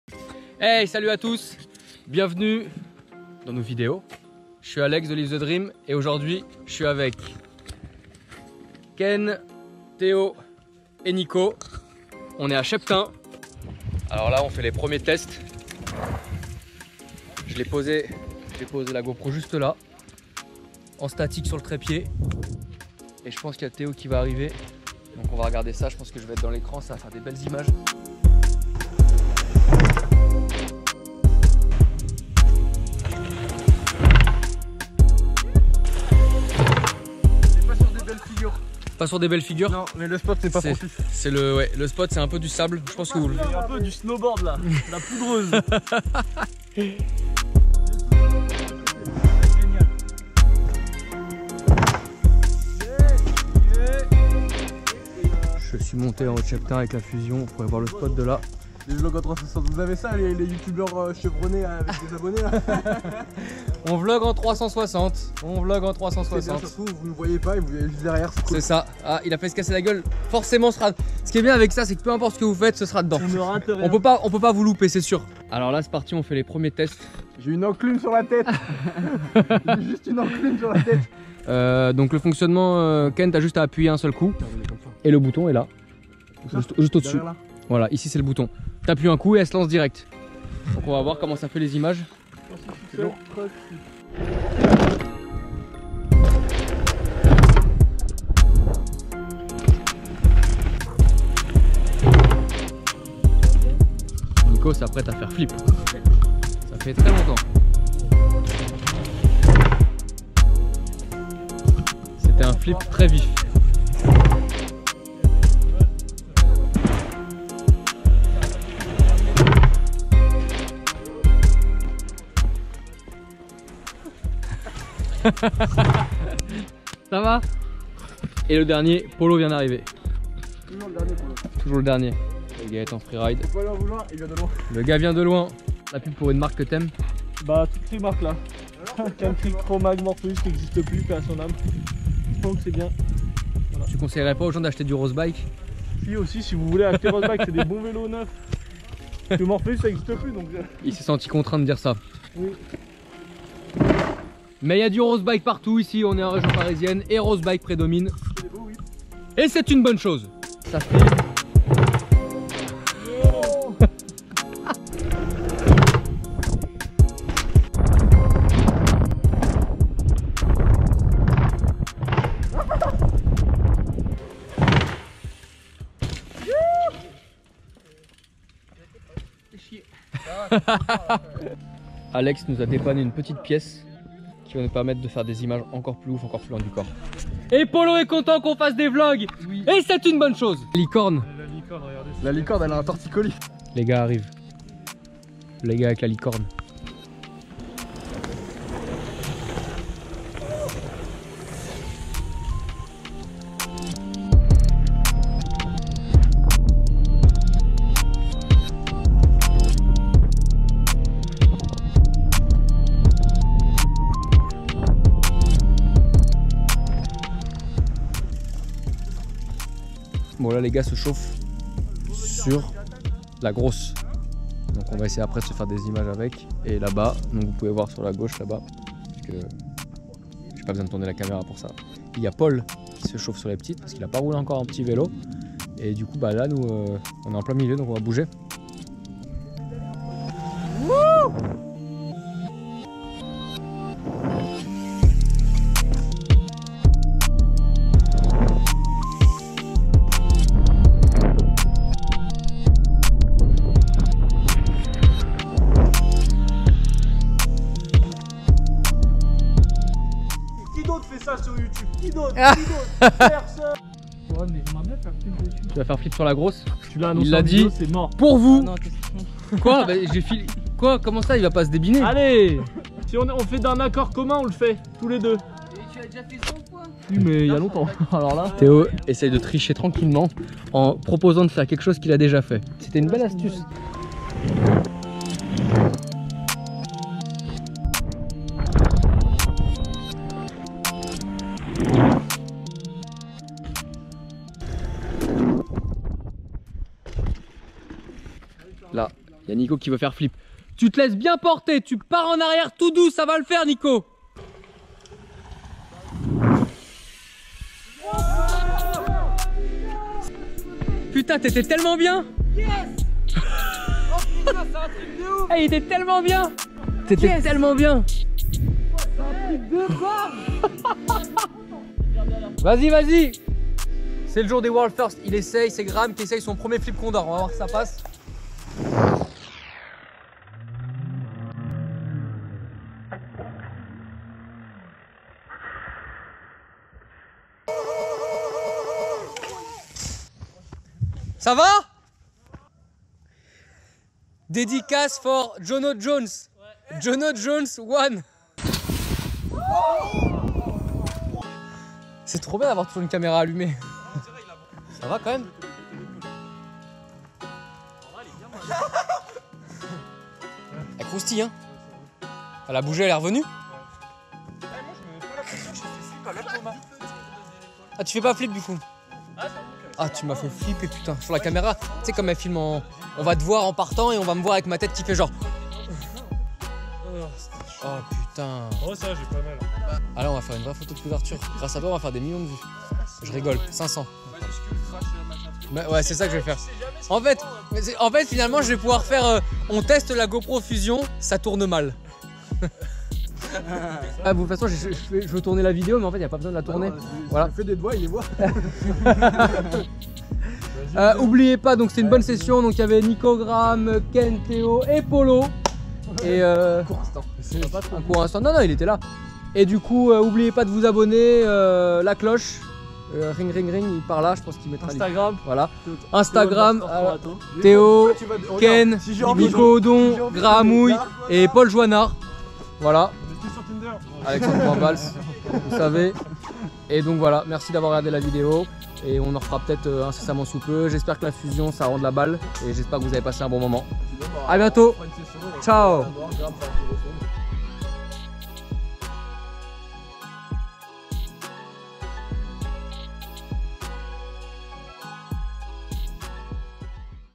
SESSION DIRT/FREERIDE A CHEPTAIN ! (ça tourne mal...)
On à voulu tester la GoPro Fusion donc on a fait une grosse session à Cheptain avec les potes !! 😉

PS: Attention aux titres aguicheurs sur YouTube 😂